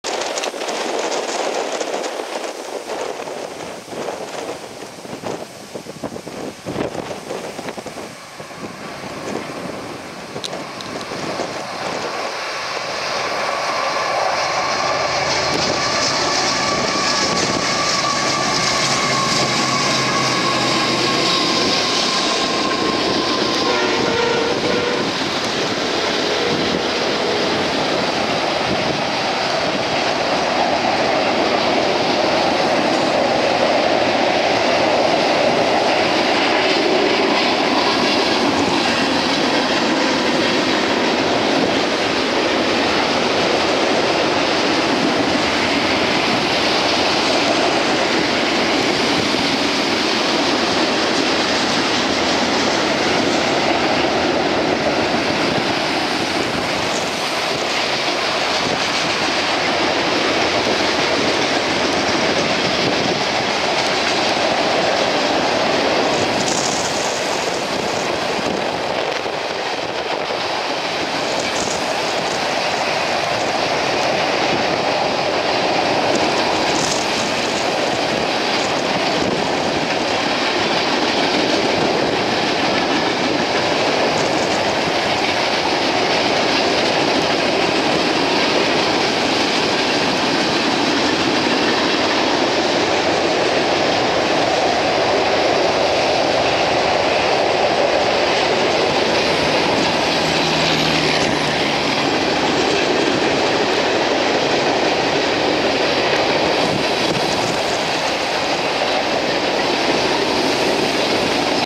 NP/BN/BNSF Powder River bridge M train.
NP/BN/BNSF Powder River bridge on a windy 16 Oct 2016. The storm clouds were rolling in as this westbound M train blew over the river. It is just 100 yards north of here where the Powder and Yellowstone meet. There is a sole grave there where the last soldier to die from Custer's battle was laid to rest.